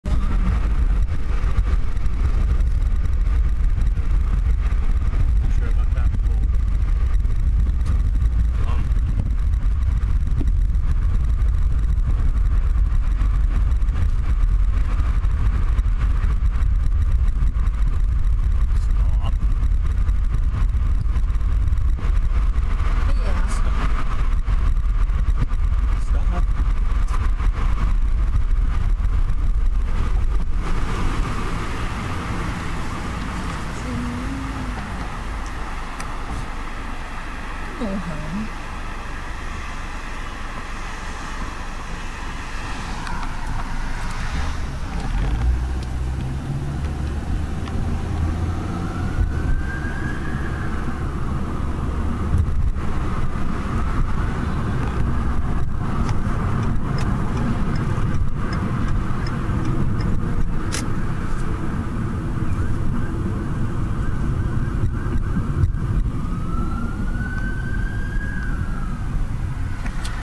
York Region Ambulance responding
Responding up Bayview ave.
Had to tell mum not to race an emergency vehicle again.